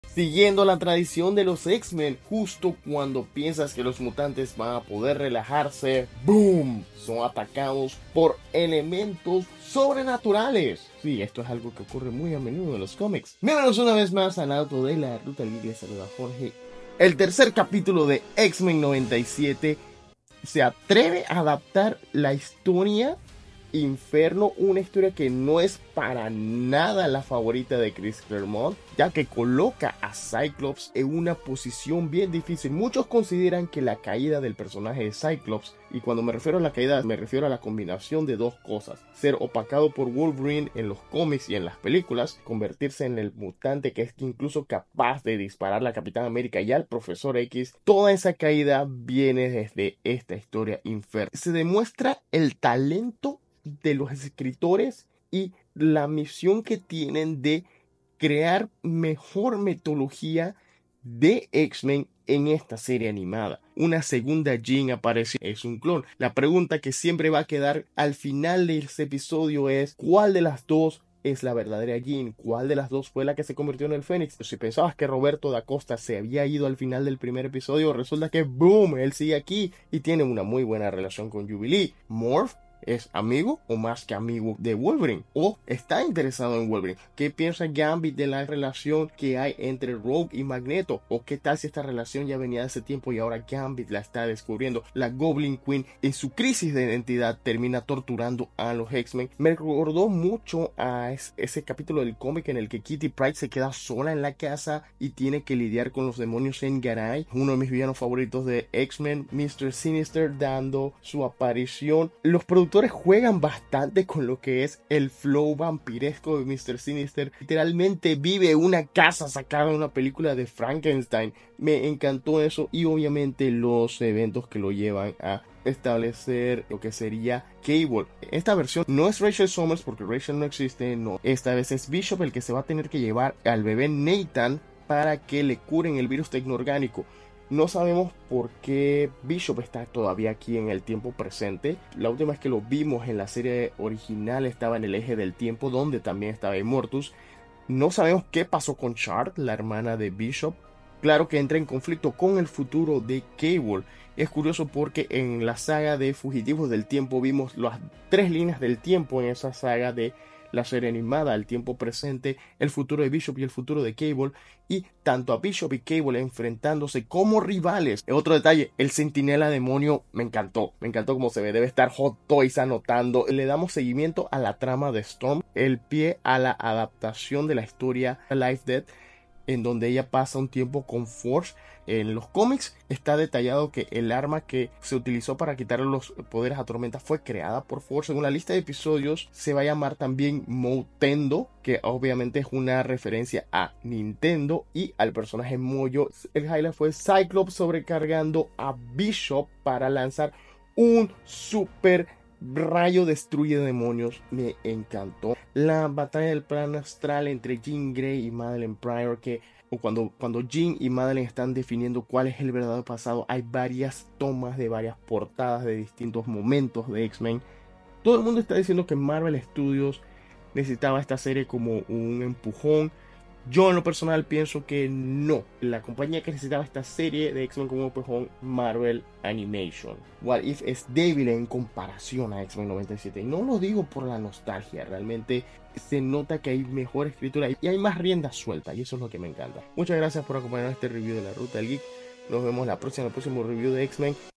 X-Men 97 adapta Inferno, explica las dos Jean y vuelve Mr Sinister | Crítica y Opinión
X-Men 97 regresa con su tercer episodio resolviendo el misterio de las dos Jean Grey y cuál es la original adaptando varias historias del cómic incluyendo Inferno prontamente. Ninguna se queda con Logan (Wolverine), la madre del bebé de Scott Summers Nathan es un clon de Jean creado por Mister Sinister.

Pero se generan más misterios dado que el episodio no contesta desde cuándo se cambiaron, es la Jean que hemos estado viendo desde el inicio un clon. Con cuál de ambas se casó Scott? Cuál tuvo el Phoenix?

También la serie prontamente infecta a Nathan Summers con el virus tecnoorgánico y lo desapacha al futuro con Bishop para convertirse en Cable.

‘X-Men 97’ es la continuación de la legendaria serie de animación ‘X-Men’ de los 90, que se estrenó el 20 de marzo de 2024 en Disney+.

En esta serie, el equipo de mutantes debe enfrentarse a nuevos peligros tras la marcha de su líder y fundador, Charles Xavier. Personajes clásicos como Cíclope, Jean Grey, Tormenta, Wolverine o Rogue deben ajustarse a esta nueva realidad sin el Profesor X que fue llevado por los Shiar y dado por muerto al final de la serie original.

El primer episodio marca un excelente comienzo para la era de los mutantes en el Universo Cinematográfico Marvel. La serie no forma parte del UCM y se sitúa fuera de la Sagrada Línea Temporal. Es canon con el Universo Cinematográfico Marvel con la excusa de los multiversos, pero por lo demás no tiene relación alguna y en su lugar se centra en continuar con la trama de la serie de los noventa.